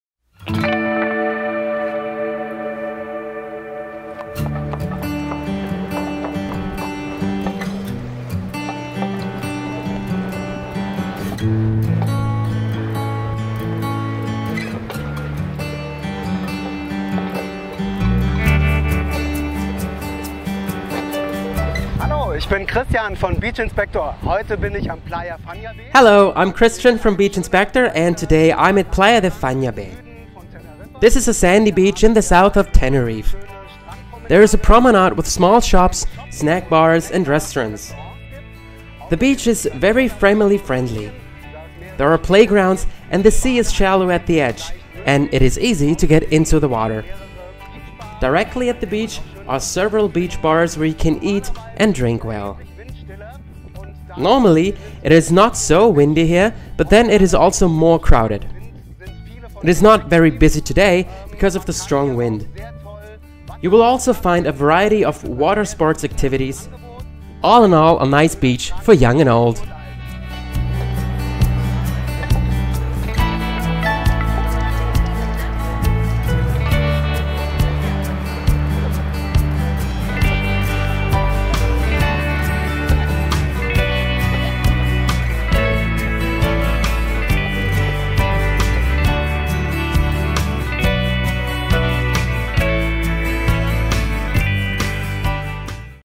Holiday in Tenerife | Beach Playa de Fañabé
Holiday in Tenerife on the most beautiful beaches in the world. Beach-Inspector shows you the dream beaches for your next holiday. Here, you will find more information about this beach:

Beach-Inspector presents to you Playa de Fañabé in Tenerife.